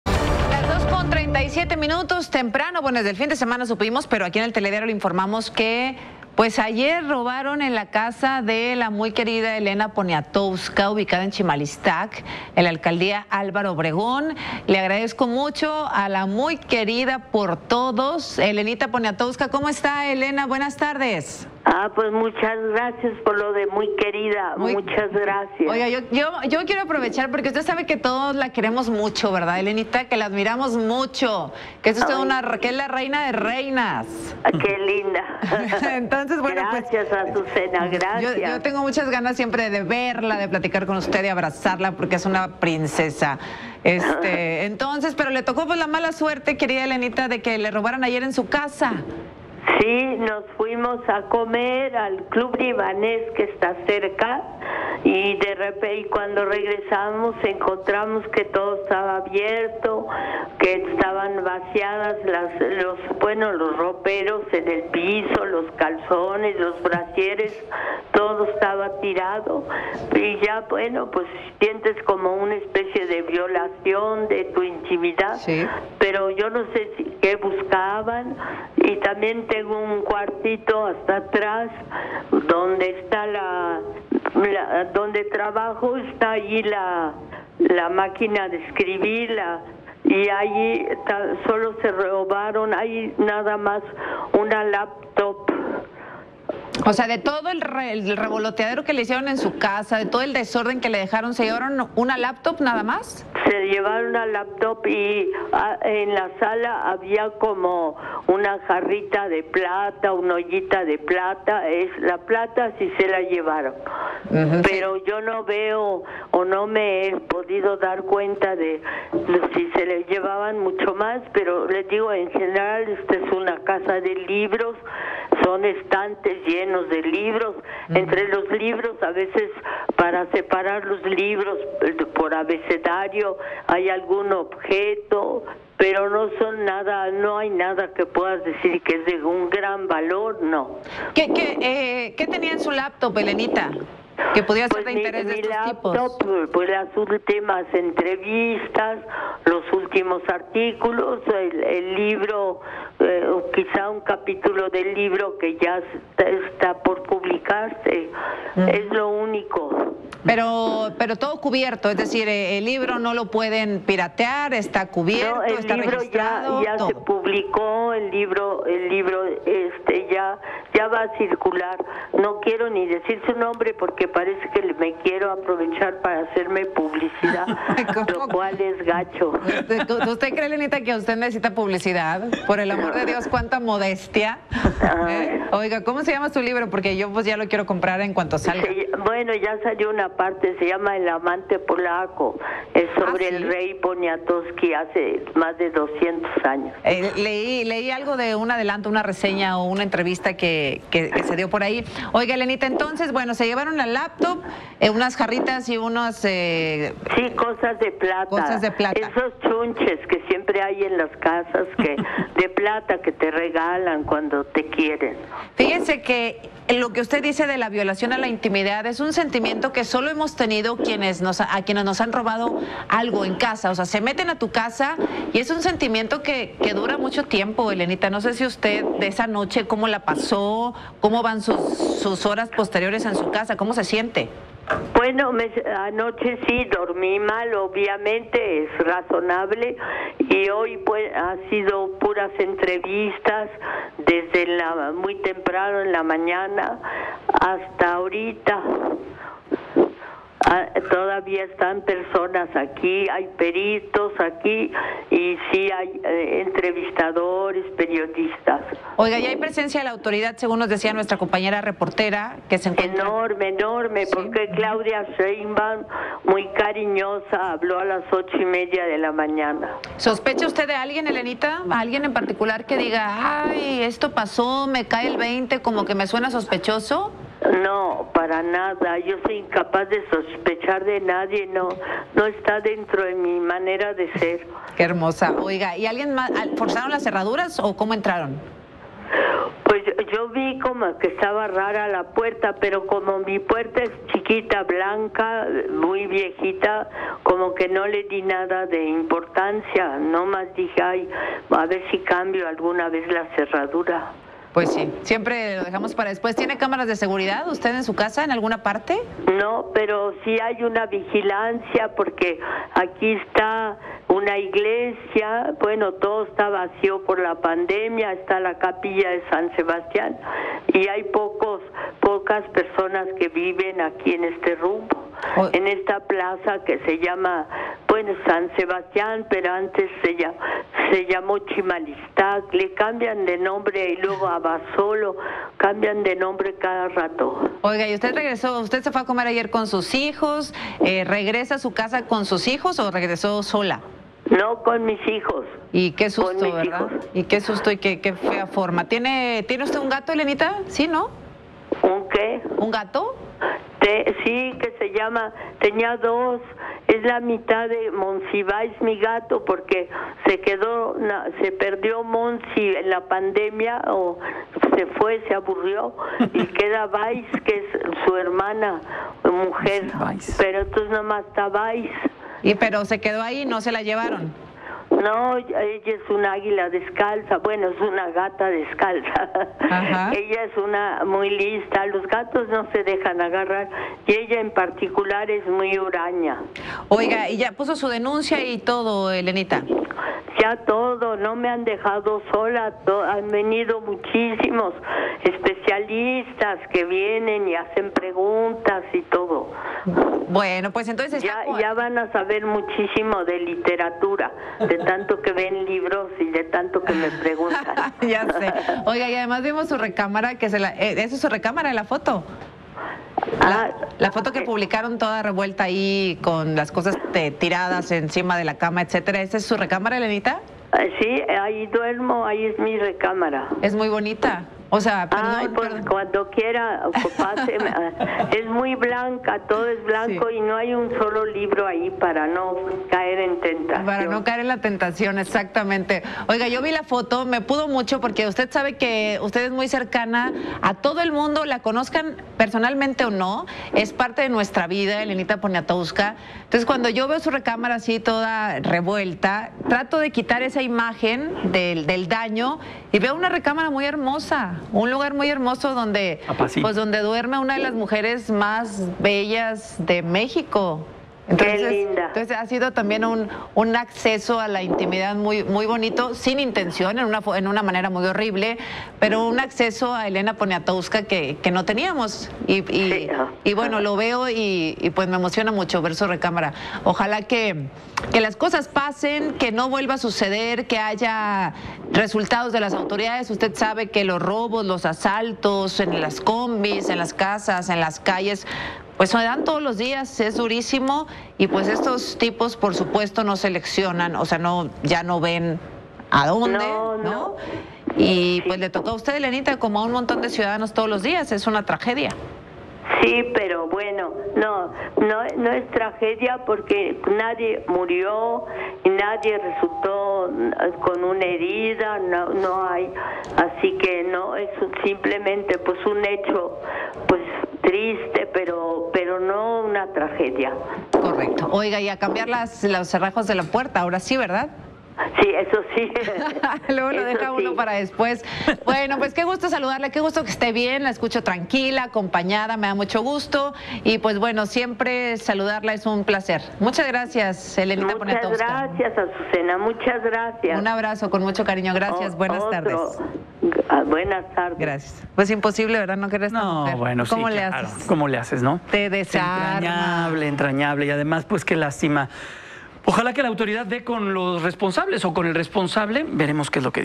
En robo a casa de Elena Poniatowska, sustraen laptop con parte de su próximo libro
Luego de que la noche del domingo se diera a conocer por personal de la Fiscalía General de Justicia de la Ciudad de México, que la casa de Elena Poniatowska fue robada, la escritora dio detalles de lo ocurrido.